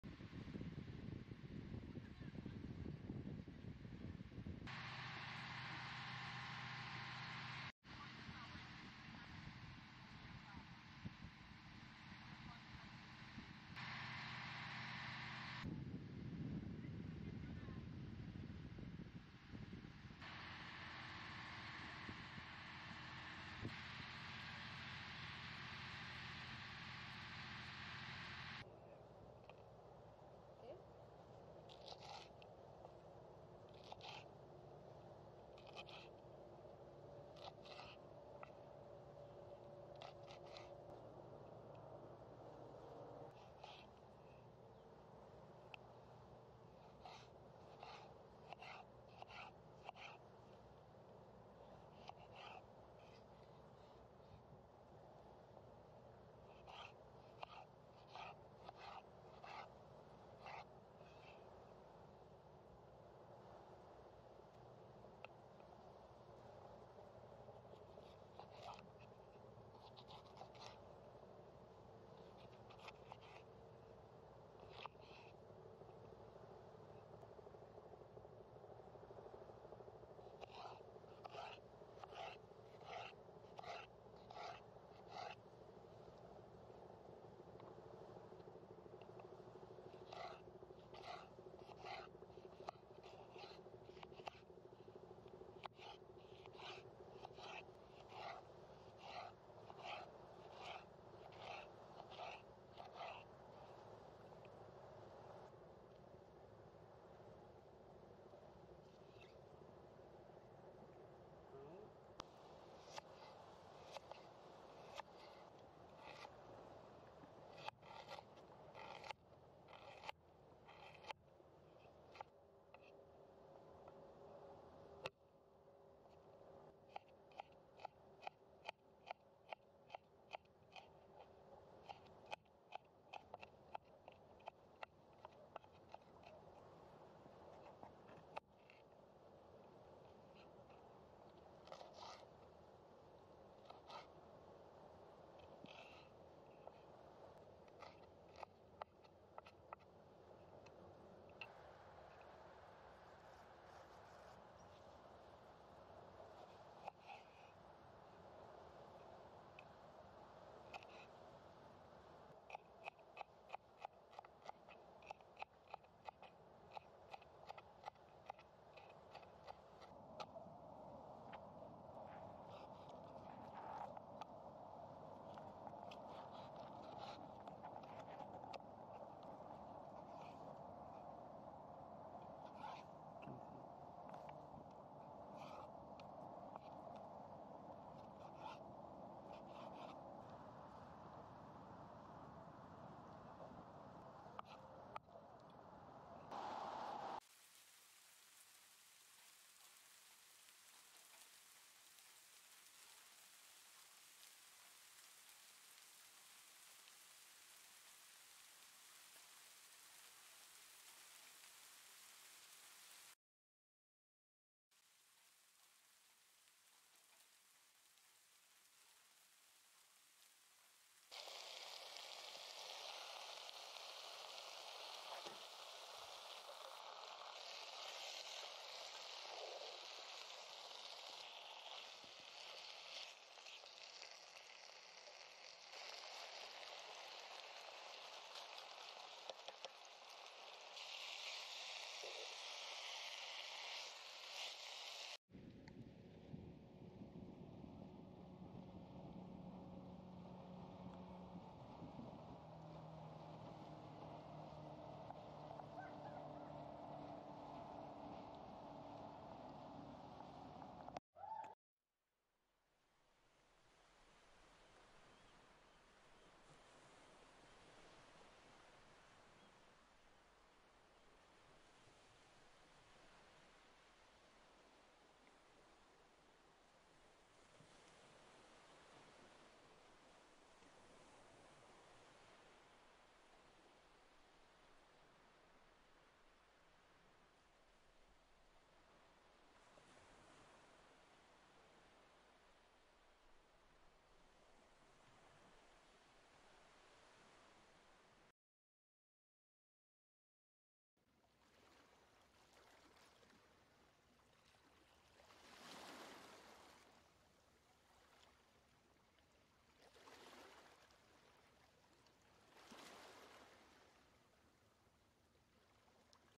Sea Side Cooking | Fajitas | Shot With GoPro Hero 11
Chicken fajitas are an easy and flavorful  meal. A classic Mexican recipe, juicy chicken is seasoned, seared and cooked to perfection, then tossed with sauteed bell peppers and onions. A squeeze of fresh lime adds a punch of flavor and the chicken fajitas are served with warm tortillas and toppings.